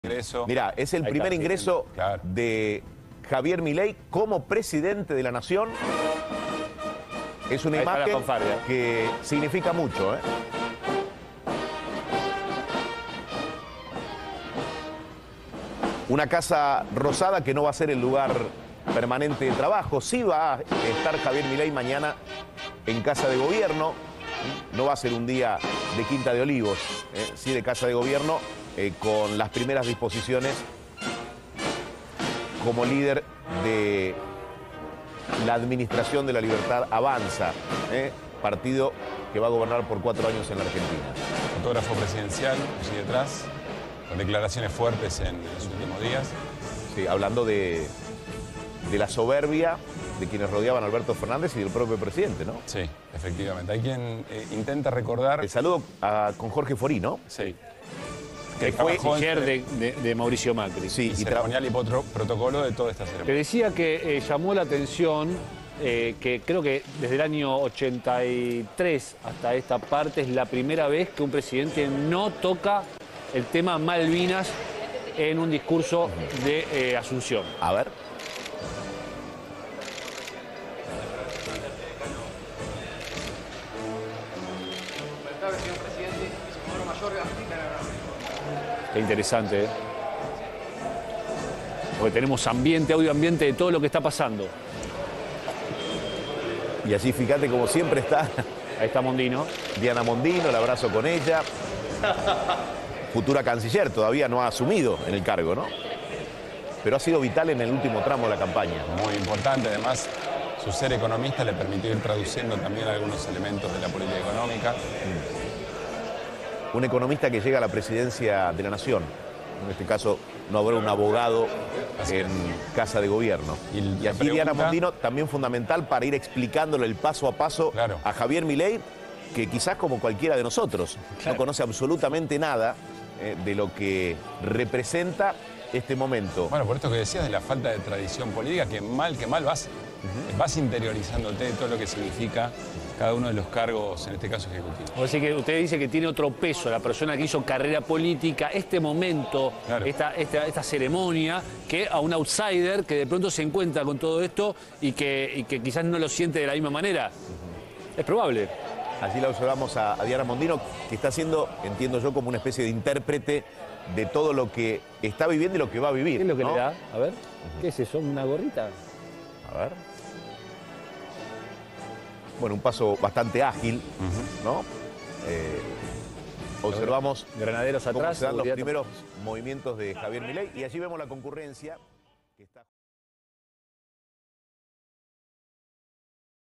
⭕ EL PRIMER INGRESO DE MILEI A LA ROSADA
Javier Milei ingresó por primera vez a la Casa Rosada junto con su hermana y la vicepresidente Victoria Villaruel.